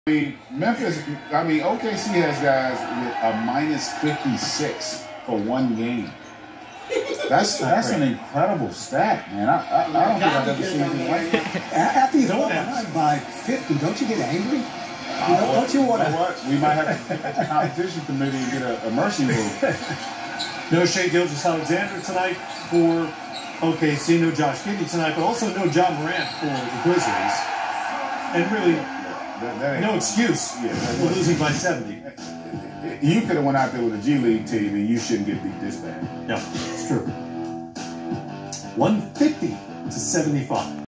OKC ONLY DOWN 75 POINTS!! 😆 🤣
Being down 20 is bad but imagine being down 75 POINTS OH MY THAT IS HORRABLE..First time in nba history a team was down by 75 points!! Crazy!